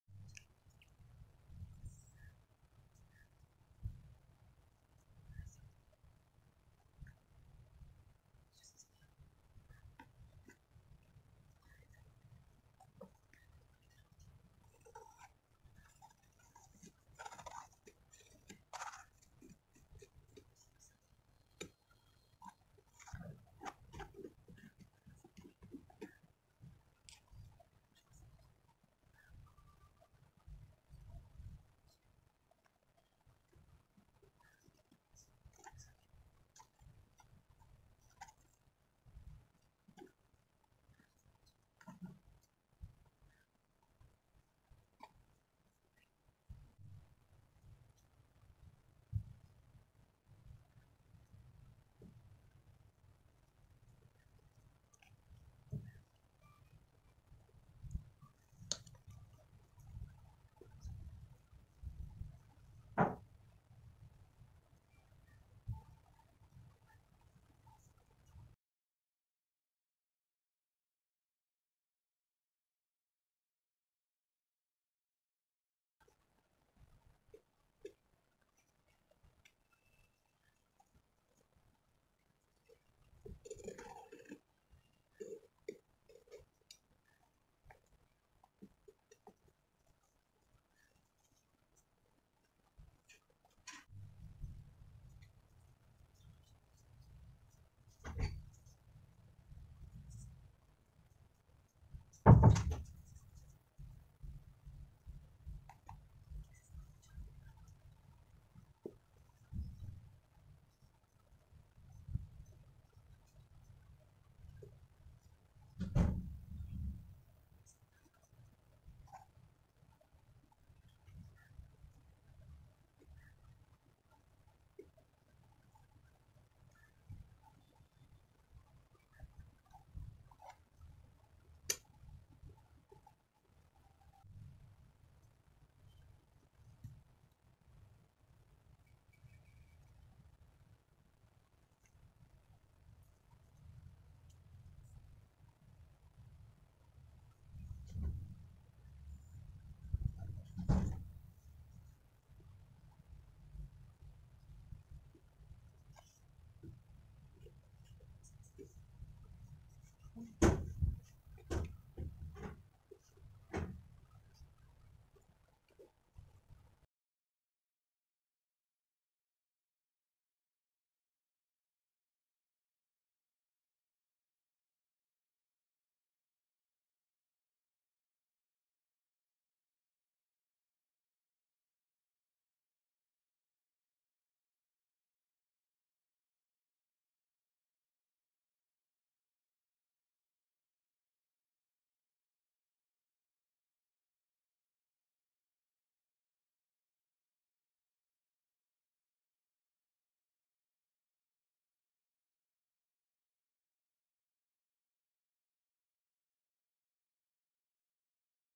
ASMR | Abstract art | Acrylic on canvas painting | Satisfying Art | Repainting | Beautiful abstract
so today i repainted and old canvas. almost 3 years old i guess :D
Acrylic on canvas using pallet e knife.